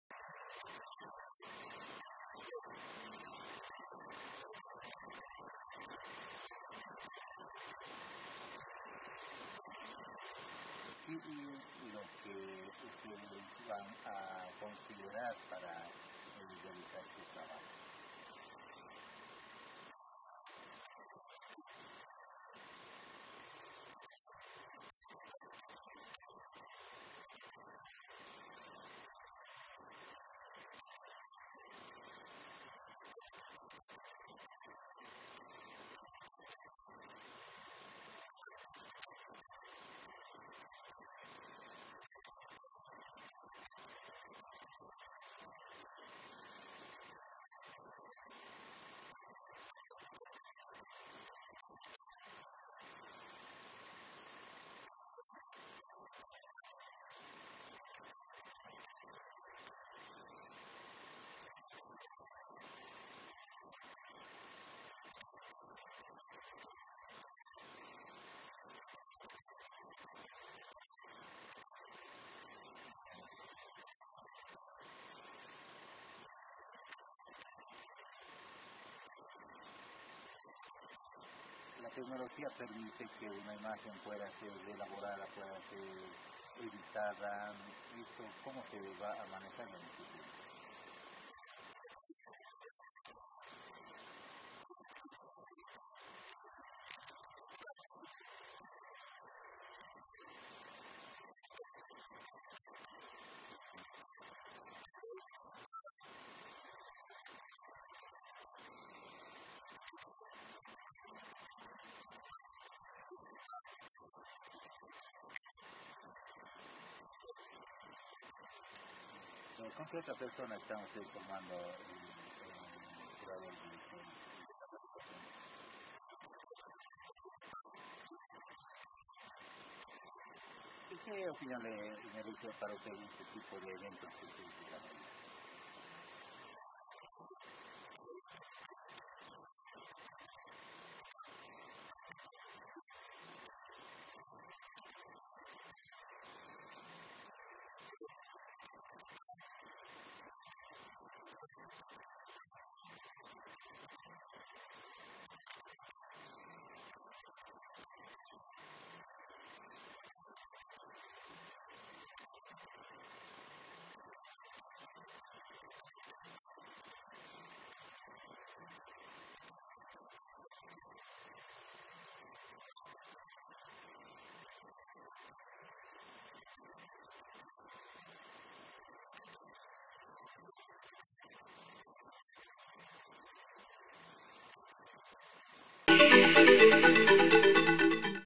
"Girón en el lente", jueces serán de Cuenca
Por buen camino continúan los preparativos para la exposición fotográfica "Girón en el lente", organizado por el Museo Casa de los Tratados, la Tercera División del Ejército Tarqui y Achiras.net, que se presentará gratutamente desde el 16 de marzo hasta el 27 de abril.
El evento esta enfocado a perennizar al cantón dentro de distintas facetas:  gente, naturaleza, tradición, arquitectura, etc., acorde al criterio del fotógrafo. Todas las personas, sean o no gironenses pueden presentar sus fotos hasta el 21 de febrero, para que éstas sean analizadas por dos fotógrafos profesionales de Cuenca, y bajo su criterio serán calificadas para ser ampliadas, pasando a formar parte de la exposición y del patrimonio del Museo, que a su vez aspira realizar exposiciones en otras ciudades.
El fotógrafo Diego Jaramillo explica detalles de su labor. Web